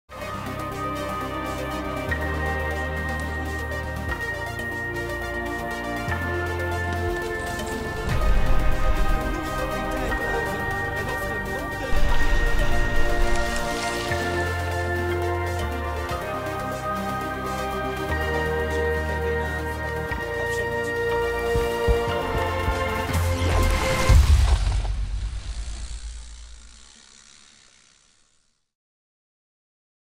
Promo NK Veldrijden 2016
In de bossen bij Hellendoorn werd het Nederlands Kampioenschap Veldrijden verreden. Bij de dames won Thalita de Jong, bij de heren was Mathieu van der Poel de sterkste.

Deze promo is gemaakt in opdracht van de organisatie van het NK Veldrijden. De promo is vertoond in de reclameblokken van RTV Oost